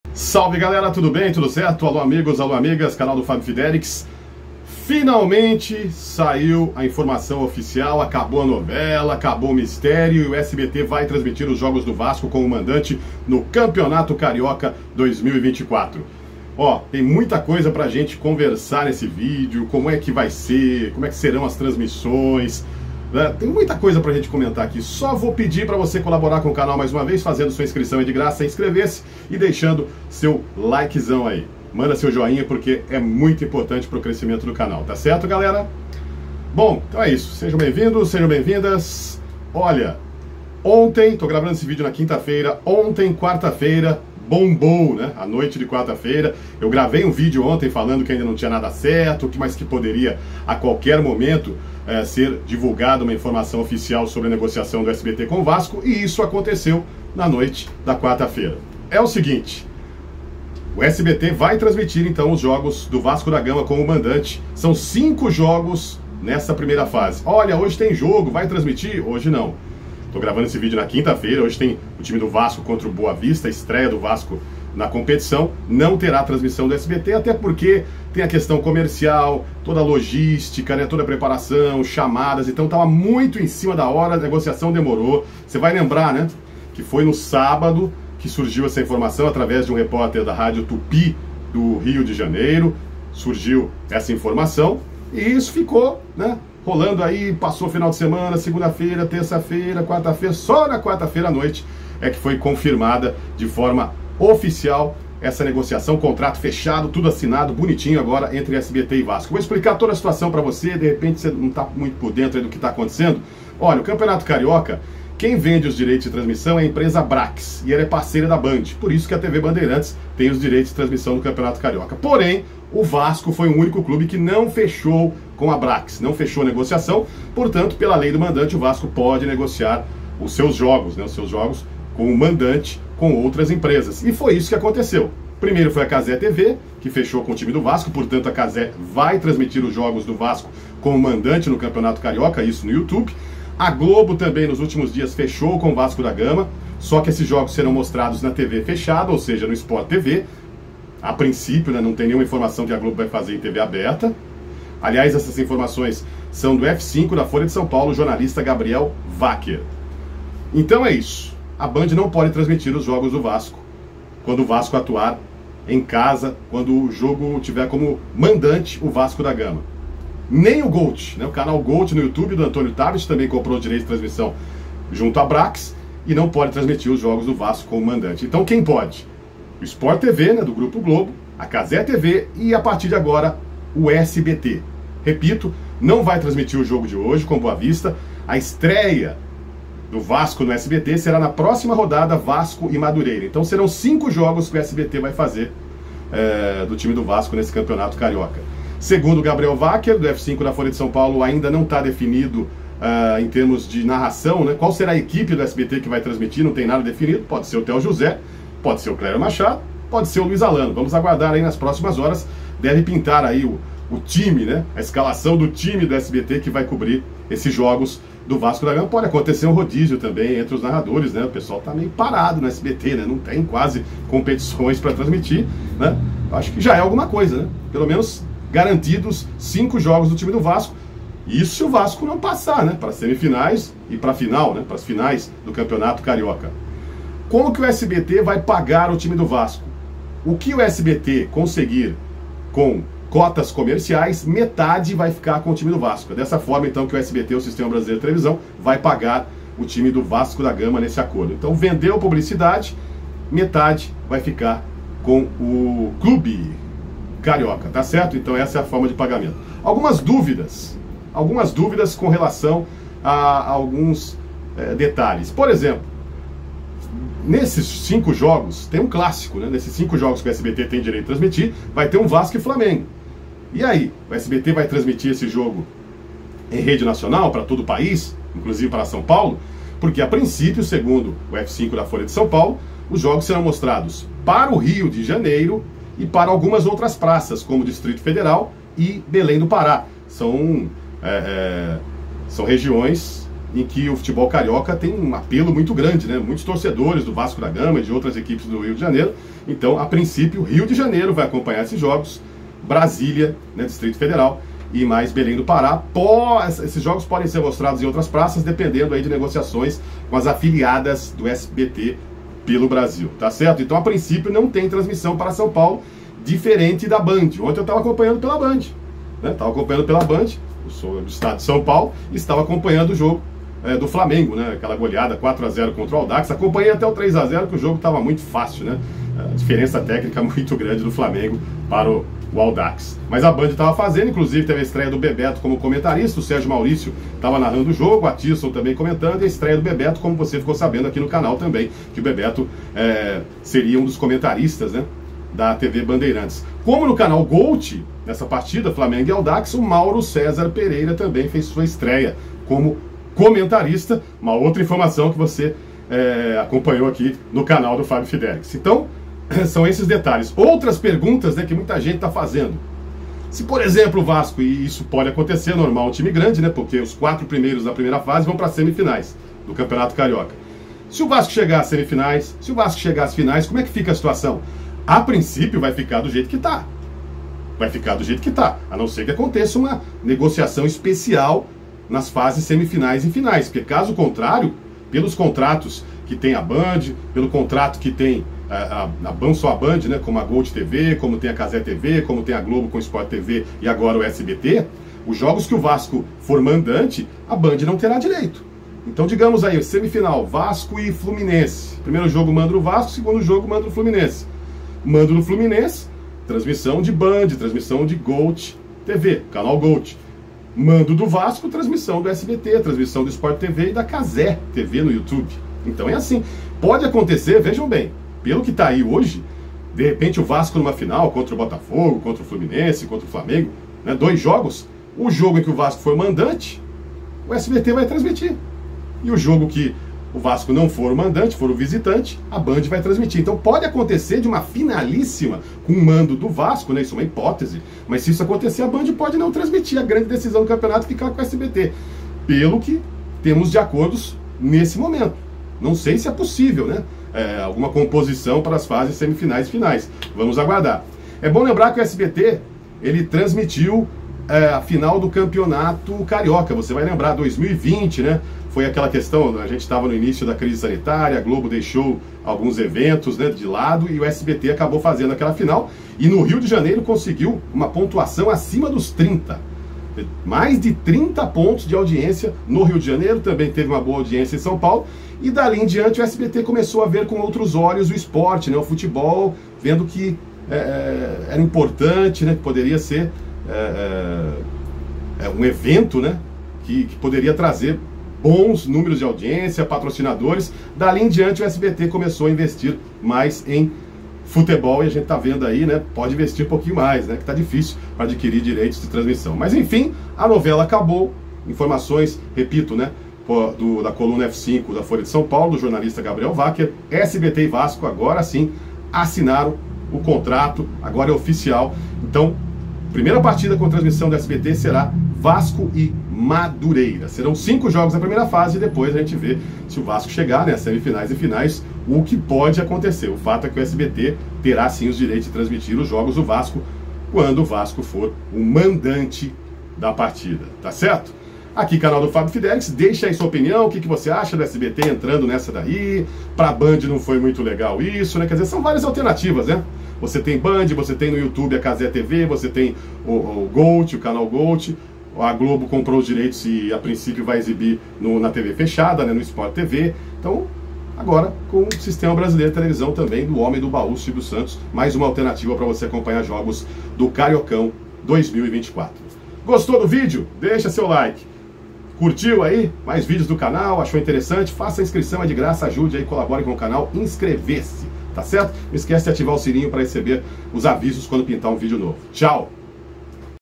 SBT VAI MOSTRAR JOGOS DO VASCO!
O SBT FECHOU CONTRATO COM O VASCO E VAI TRANSMITIR PARTIDAS DA EQUIPE CARIOCA.